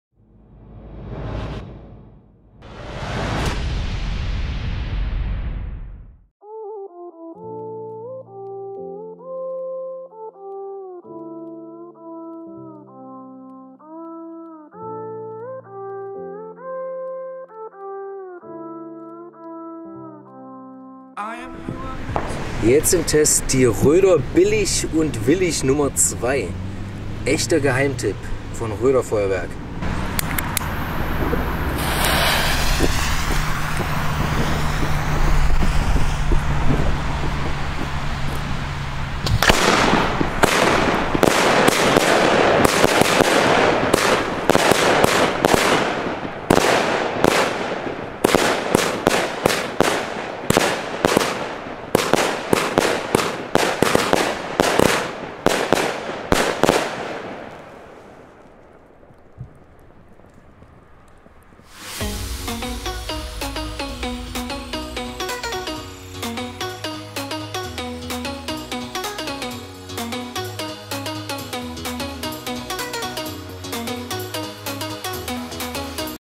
Röder Billich und Willich No 2 | Batterie für 10 € | Der Preishammer | Günstig, aber edel! [Full HD]
Heute im Test: Röder Billich und Willich

Für nen "Zehner" ist das eine absolut empfehlenswerte Batterie!

* Kamera, Camcorder, Videoequipment:

*2x "3-Flammen-Zigarrenbrenner"

* 2x BESTER Brenner (Sturmfeuerzeug)

KEEP WATCHING.